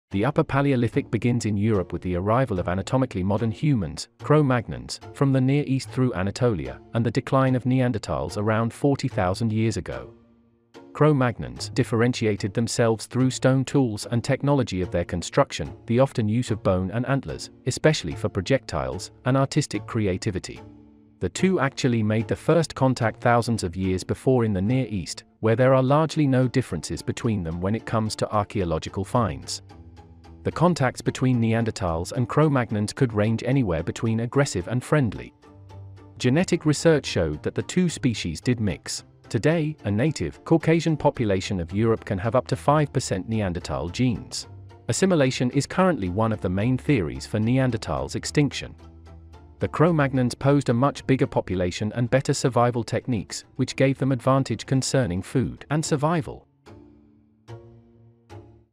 Cro-Magnons in the Heart of Europe - First Anatomically Modern Humans
The upper paleolithic begins in Europe with the arrival of anatomically modern humans, Cro-Magnons, from the Near East through Anatolia, and the decline of Neanderthals around 40,000 years ago.

Cro-magnons differentiated themselves through stone tools and technology of their construction, often use of bone and antlers (especially for projectiles), and artistic creativity. During this time, Cro-Magnons created the first cave paintings of animals (the Chauvet cave), the first painted stones (Fumane cave and Hohle Fels cave) and the first proper figurines (Venus from Hohle Fels, man-lion from Hohlenstein-Stadel cave, and more animal figurines from Vogelherd cave).

The human remains and fossils of the earliest part of the upper paleolithic (the Aurignacian) are rare finds, the oldest being Peștera cu Oase caves (Romania). While graves were found in the Cueva Morin cave (Spain), the human remains inside have completely disintegrated. In Slovenia, the prominent Aurignacian sites are Špehovka cave, Potočka zijalka, Mokriška cave and Divje babe.

The arrival of Cro-Magnons actually accelerated the cultural and technological progress of them and the Neanderthals. The two actually made the first contact thousands of years before in the Near East, where there are largely no differences between them when it comes to archeological finds. One was making the same tools as the other.

The contacts between Neanderthals and Cro-Magnons could range anywhere between aggressive and friendly. Genetic research showed that the two species did mix. Today, a native, caucasian population of Europe can have up to 5% Neanderthal genes, though this usually ranges between 2 and 3 percent.

Assimilation is currently one of the main theories for Neanderthals’ extinction. The Cro-Magnons posed a bigger population and better survival techniques, which helped with food supply, fertility and the survival rate.